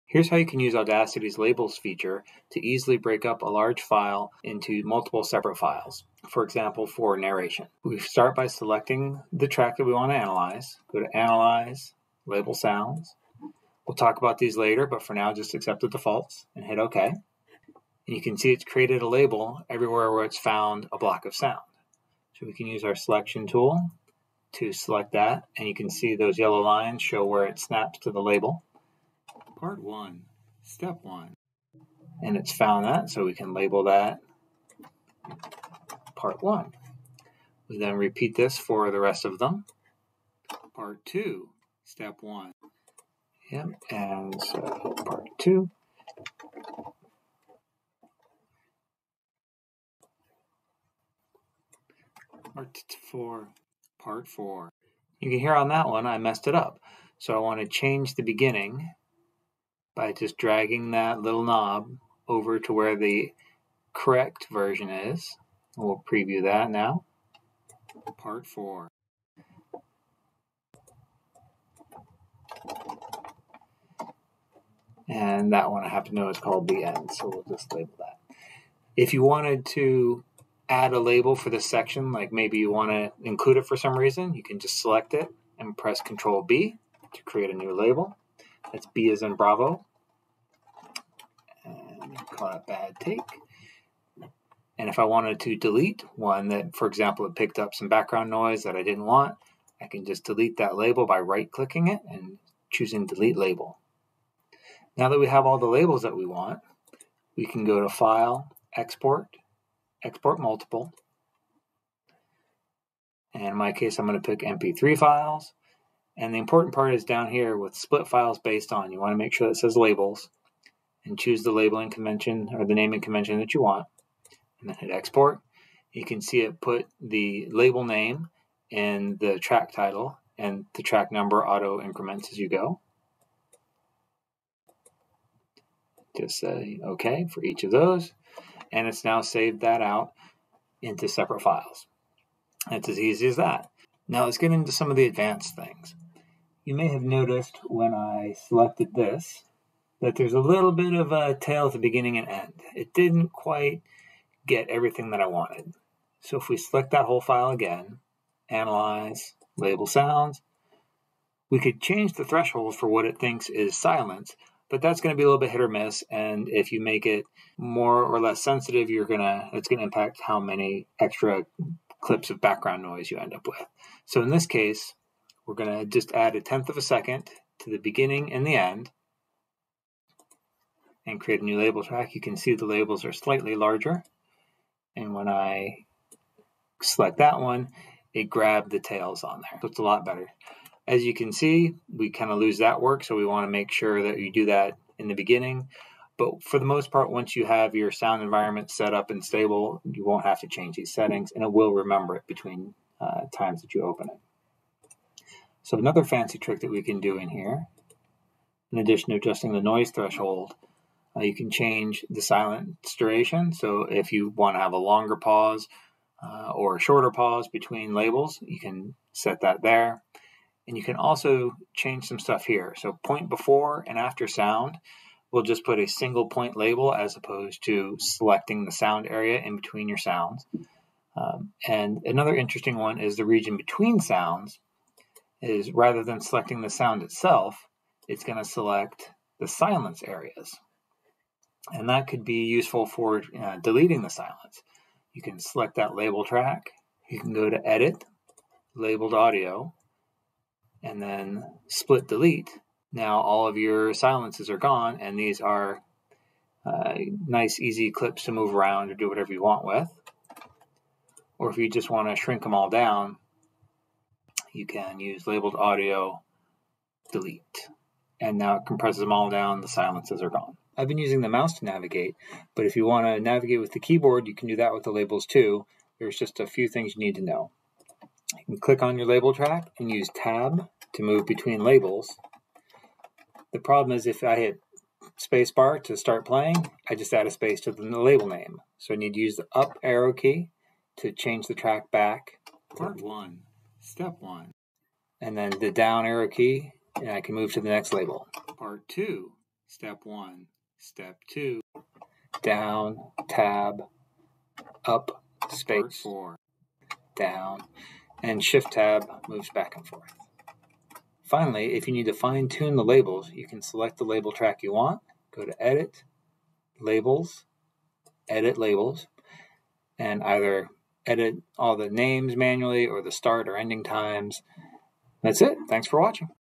Audacity labels for narration
Use Audacity's label sounds feature (sound finder in versions below 3.0.0) to quickly and easily break up a large file.  Great for splitting up narration!